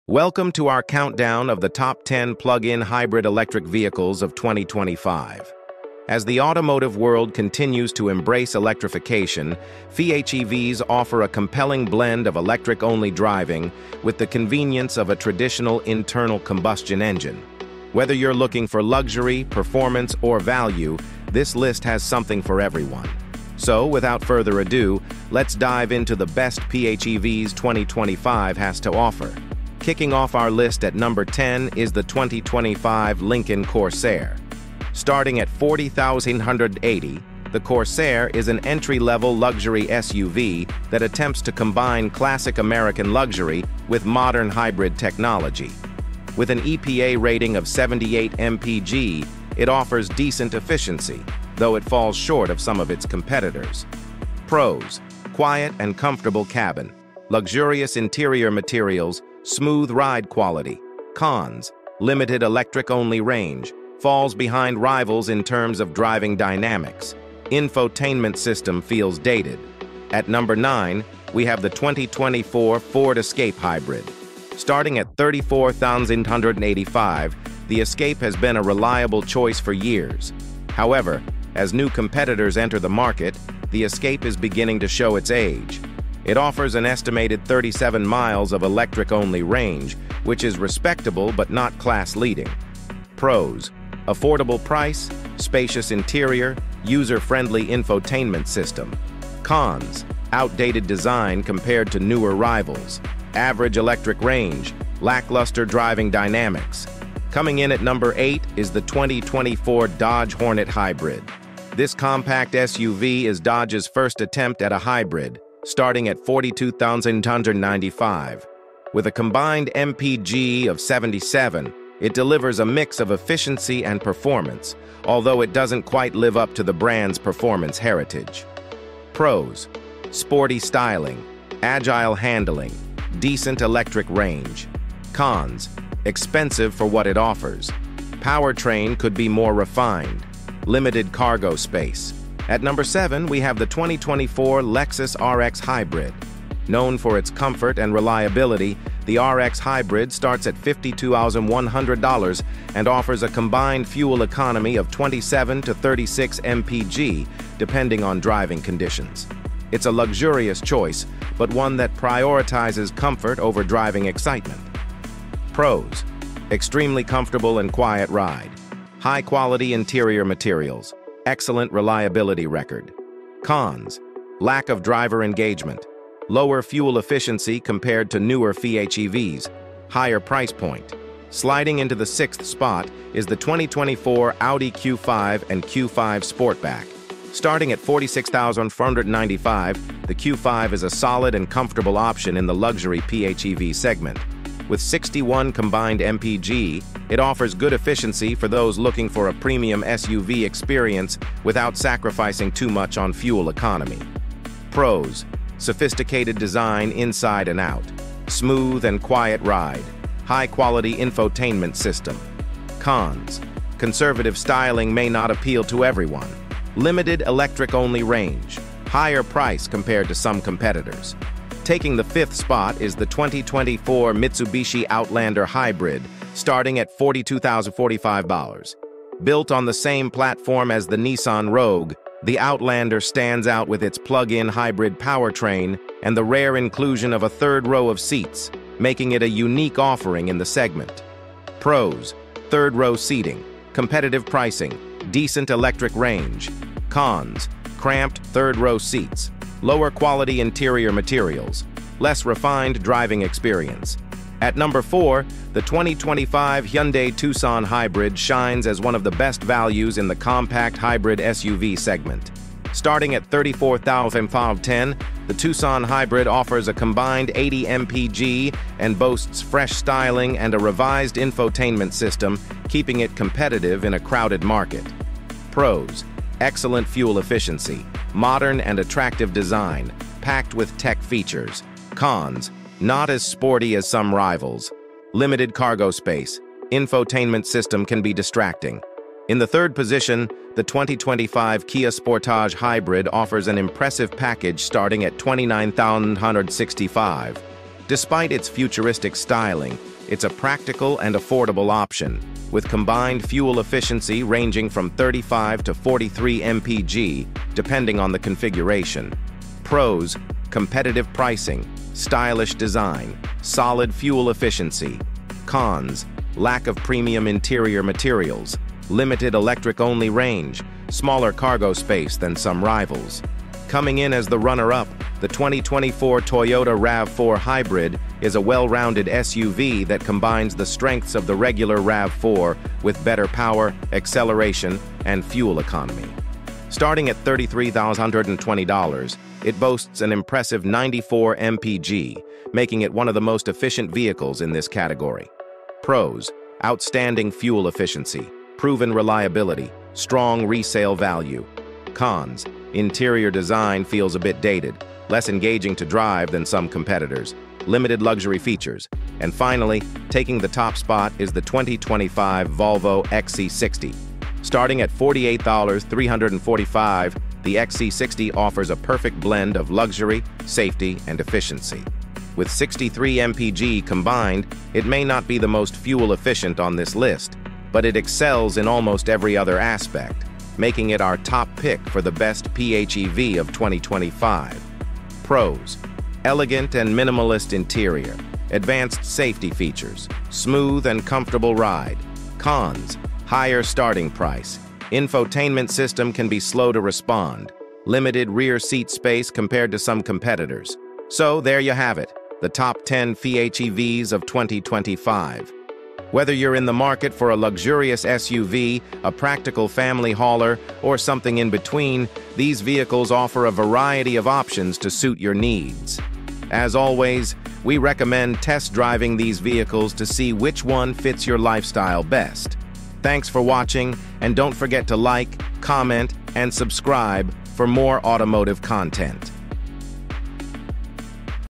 Top 10 Plug In Hybrid Electric Vehicles 2024 and 2025 | Choose the Best!
Welcome to our countdown of the top 10 plug-in hybrid electric vehicles of 2025! In this video, we explore the best PHEVs on the market, from entry-level luxury to high-performance models. Whether you're looking for value, style, or eco-friendly power, these vehicles offer a blend of electric efficiency and traditional engine convenience. From the 2025 Lincoln Corsair to the Volvo XC60, we'll guide you through the pros and cons of each model to help you make an informed decision.